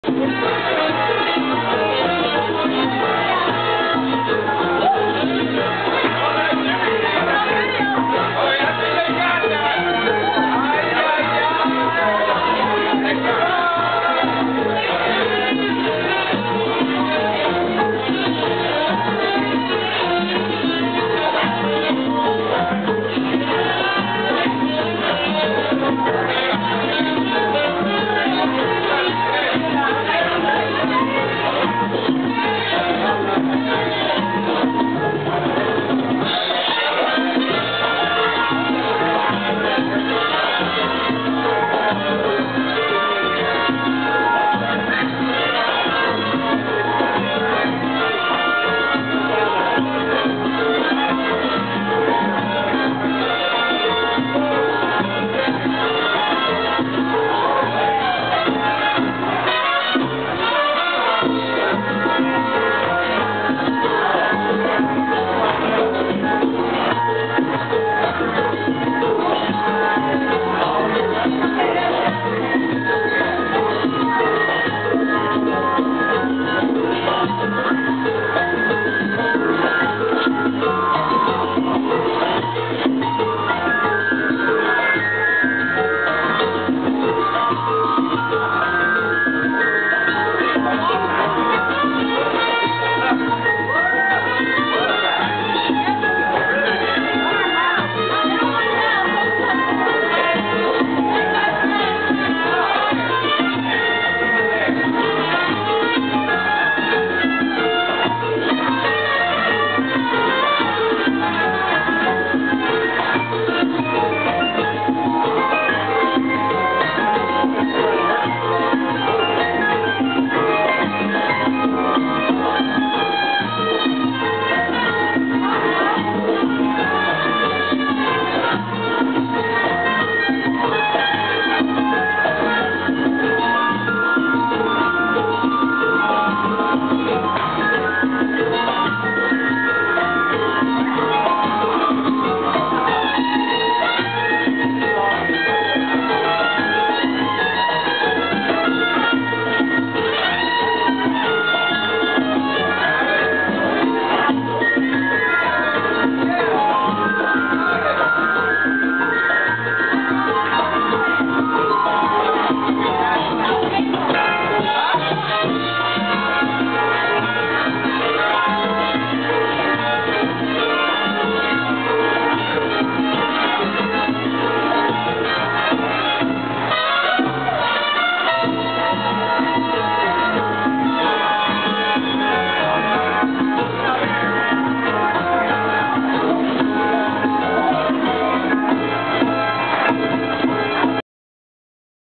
Luz Rodriguez & Joe Garcia @Mr. Mambo's
Luz Rodriguez & Joe Garcia at Mr. Mambo's Magna Gopal Event May 14, 2011.